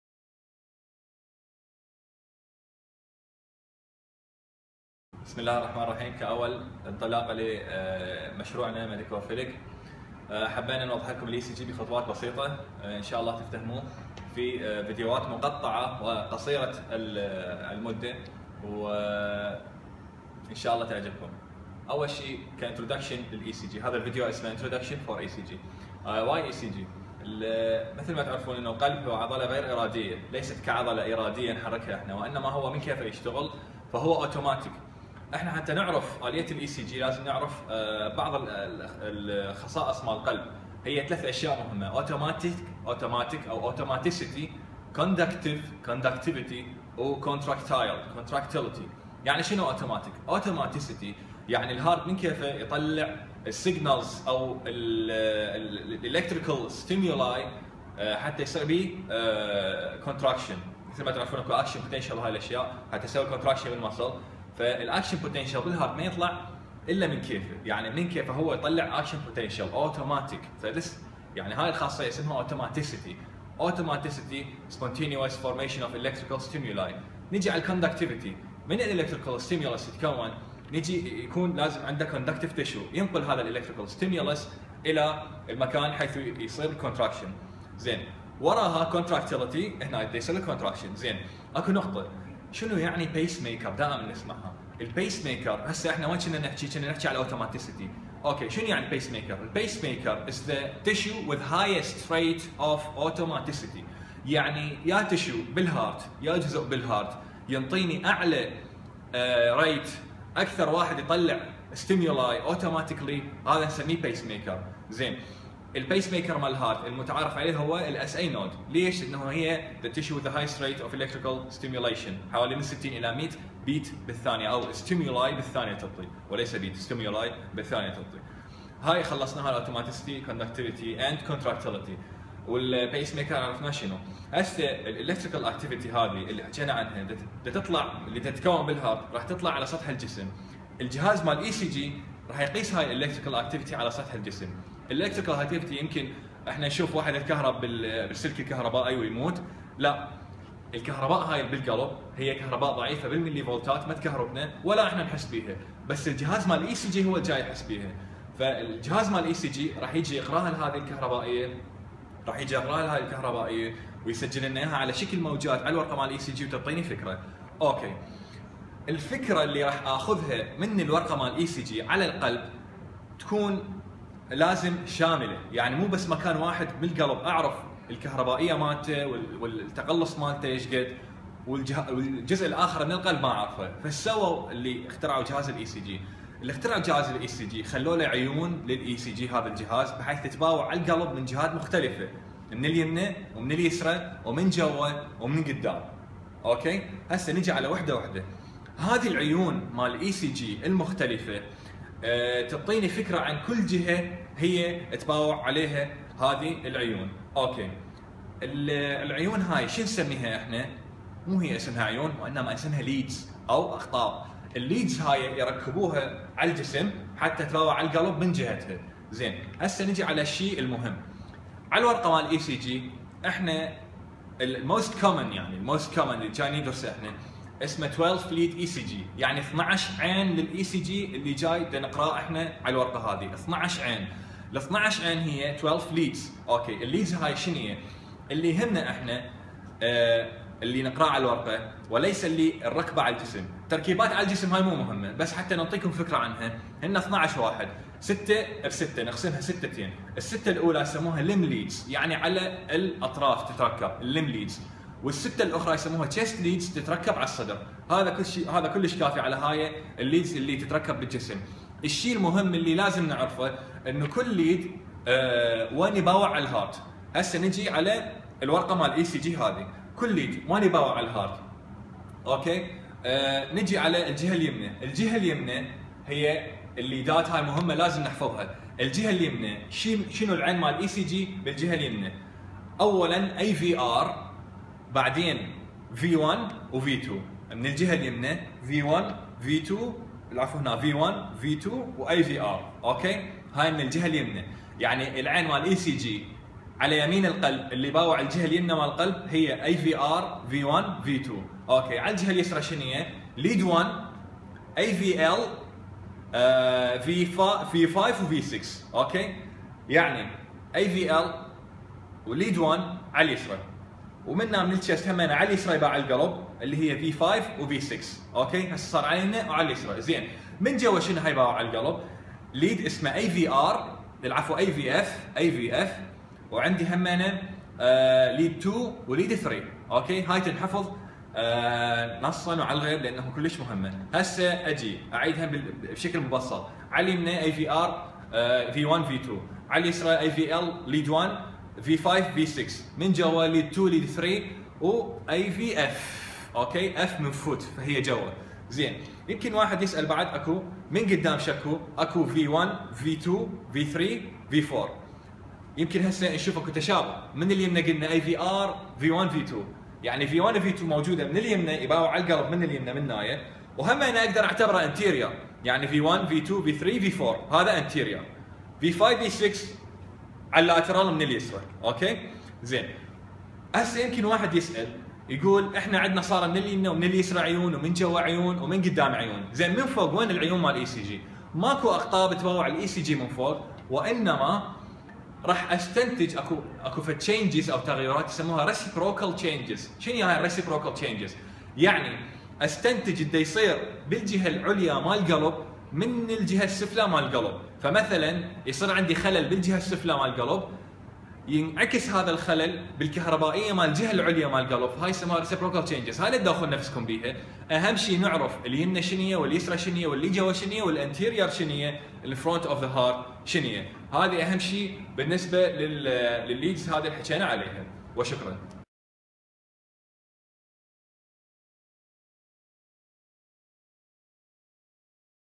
Introduction To ECG - ECG 1
Description: ( please note : SA node rate is 60-100 / minute ( not second ) ) ...
International Federation of Medical Students Association ( IFMSA ) -  Iraq / Standing Committee on Medical Education ( SCOME ) ...